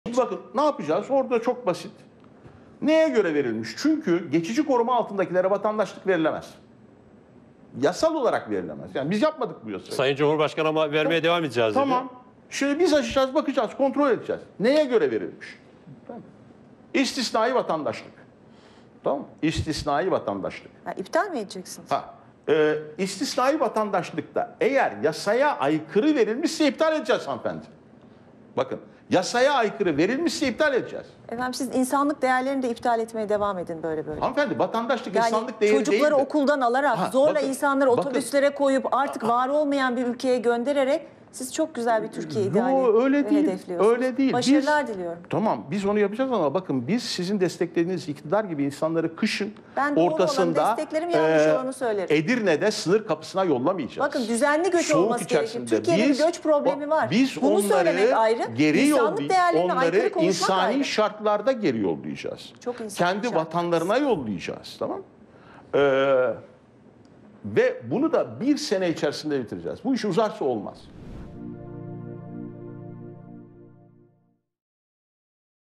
رئيس حزب تركي معارض يتوعد بسحب الجنسية التركية من السوريين!
يمكنكم متابعتنا عبر وسائل التواصل الاجتماعي المختلفة:
فيس بوك: